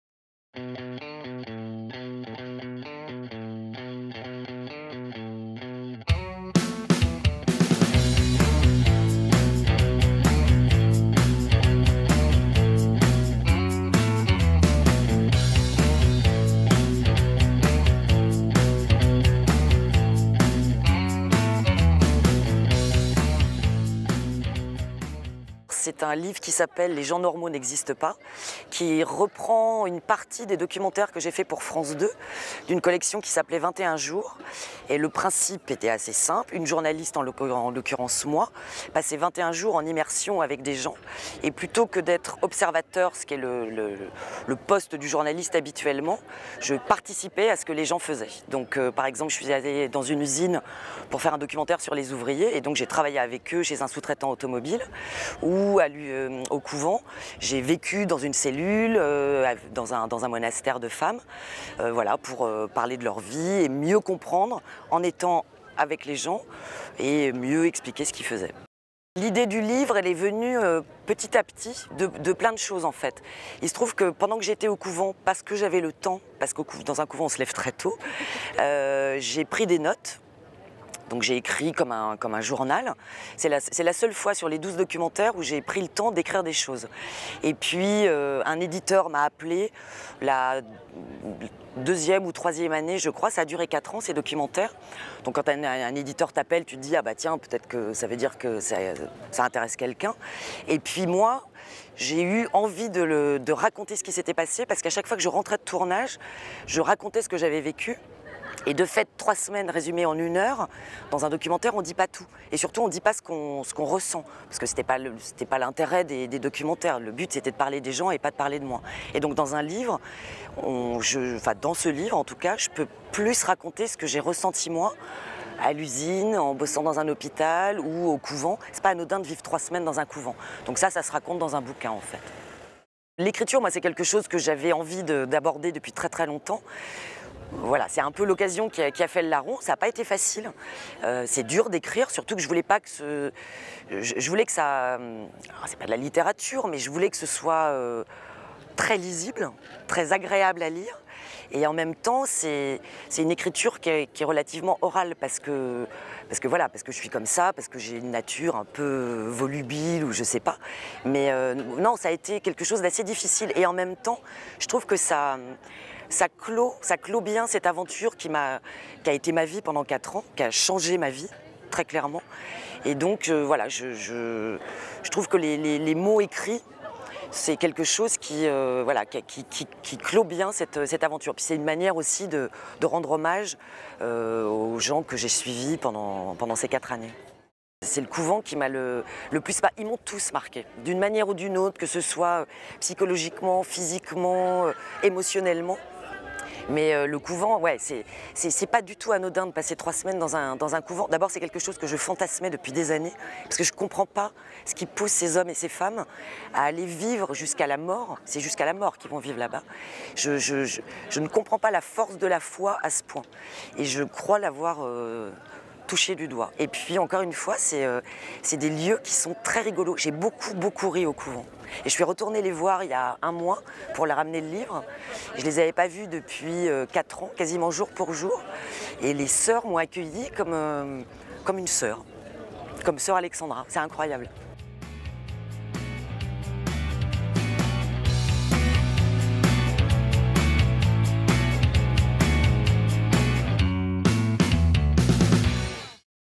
Alexandra Alévêque - Les gens normaux n'existent pas : chroniques de 21 jours
À l'occasion du festival "Étonnants voyageurs" de Saint Malo, Alexandra Alévêque vous présente son ouvrage "Les gens normaux n'existent pas : chroniques de 21 jours" aux éditions Robert Laffont.